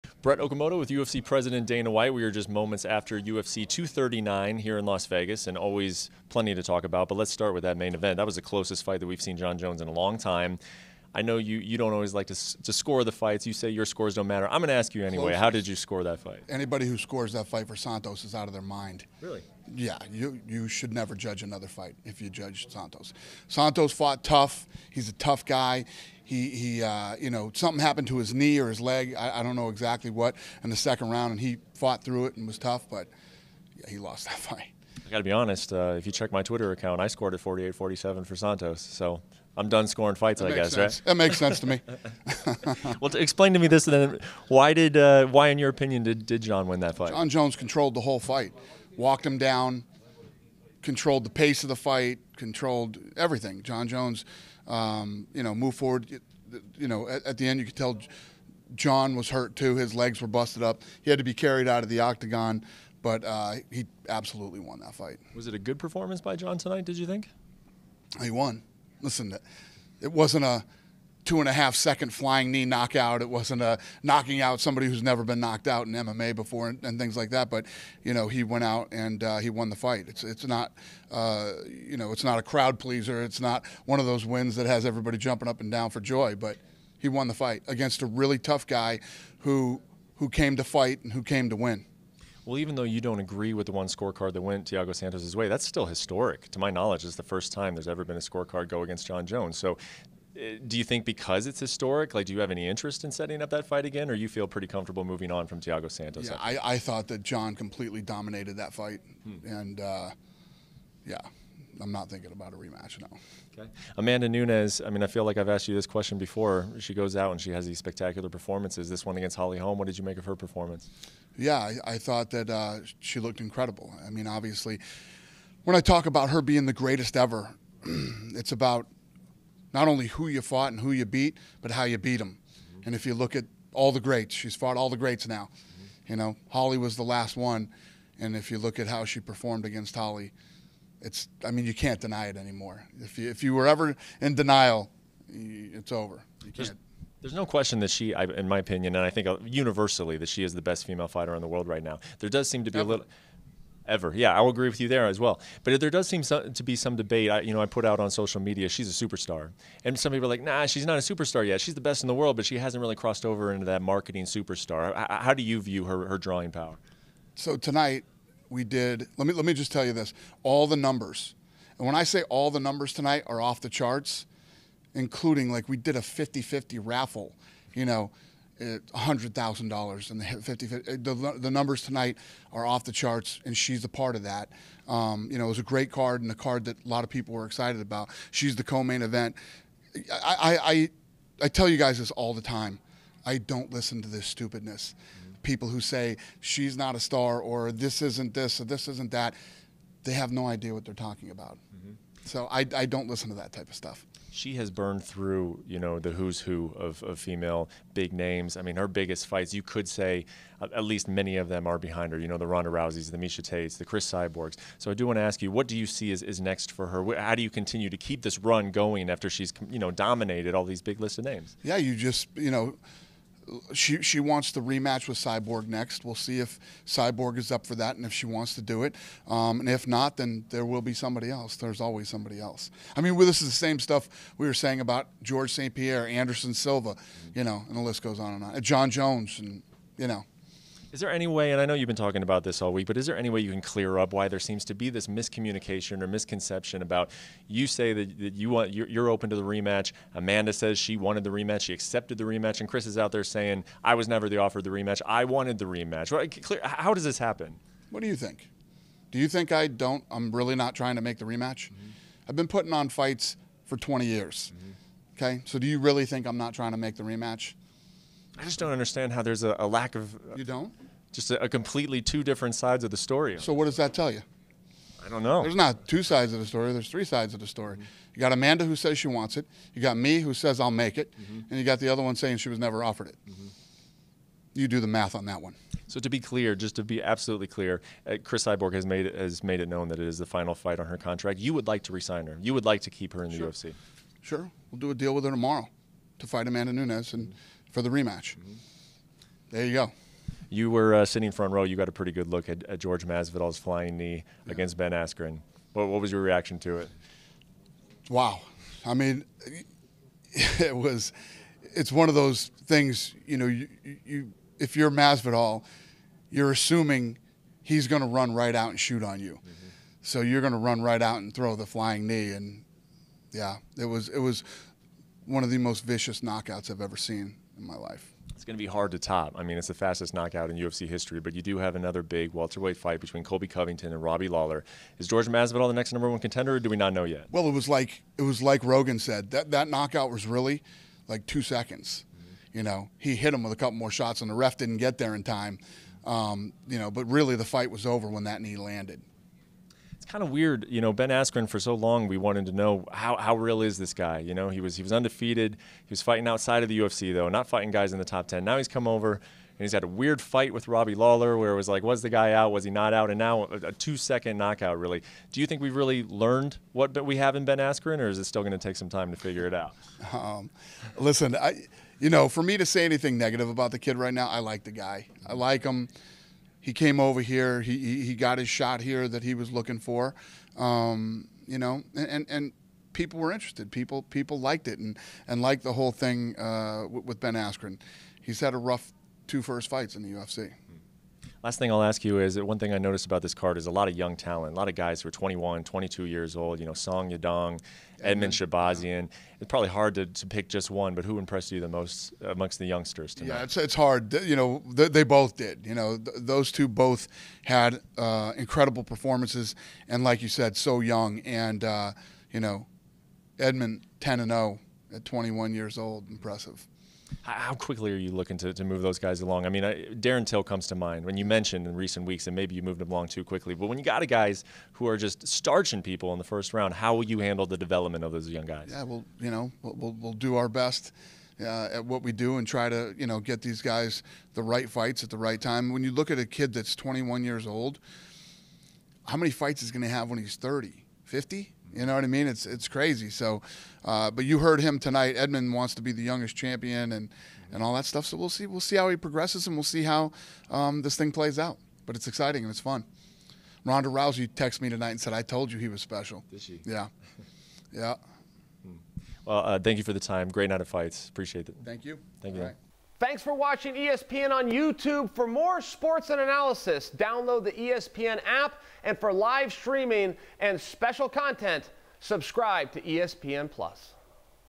Dana White: Jon Jones 'completely dominated' Thiago Santos | UFC 239 | ESPN MMA
Dana White says "anybody who scores that fight for Santos is out of their mind." White says Jon Jones controlled the whole fight vs. Thiago Santos at UFC 239. White continues that no one can deny that Amanda Nunes is the best woman fighter of all time after her knockout win vs. Holly Holm. White says Nunes wants the rematch with Cris Cyborg and if Cyborg also wants the rematch then it will happen. White calls Jorge Masvidal's knockout of Ben Askren "one of the most vicious knockouts" he has seen in his life. White finishes by saying the young fighters at UFC 239 like Song Yadong will be exciting for the future of the UFC.